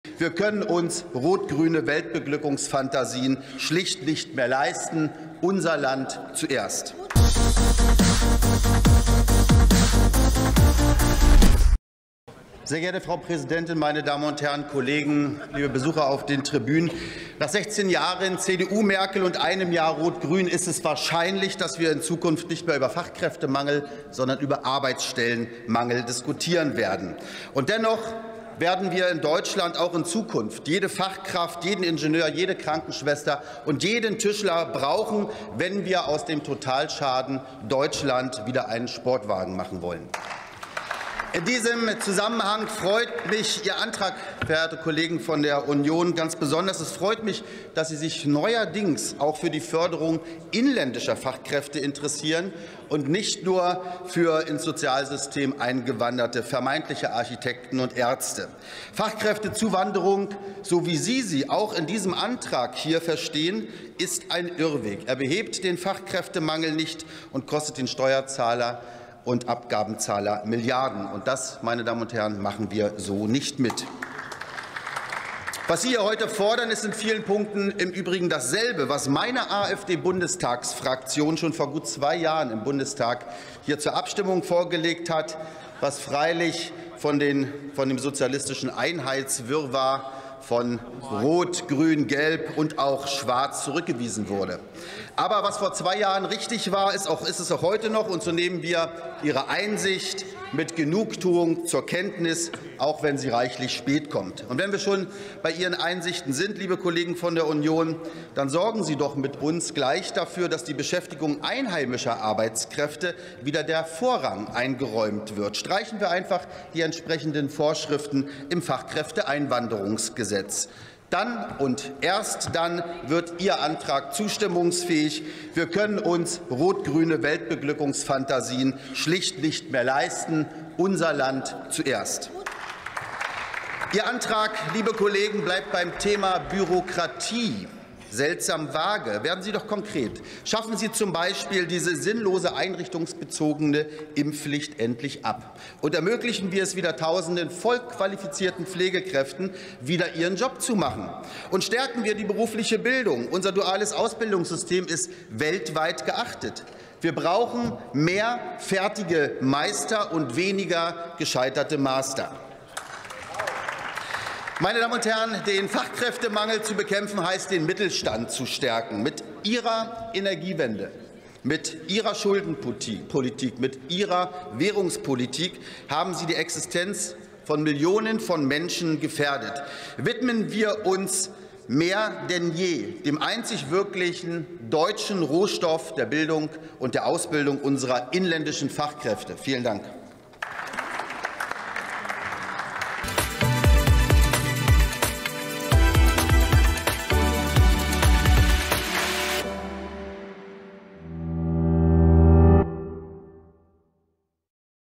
Wir brauchen mehr Meister als gescheiterte Master! | Enrico Komning MdB
Heute wurde im Deutschen Bundestag über einen Antrag der CDU/CSU-Fraktion zum Fachkräftemangel debattiert. Mit ihrem Antrag "Fach- und Arbeitskräfte mit zielgerichteten Maßnahmen im Inland gewinnen" interessiert sich die Union neuerdings für inländische Fachkräfte. Gibt es plötzlich Einsicht bei der CDU/CSU?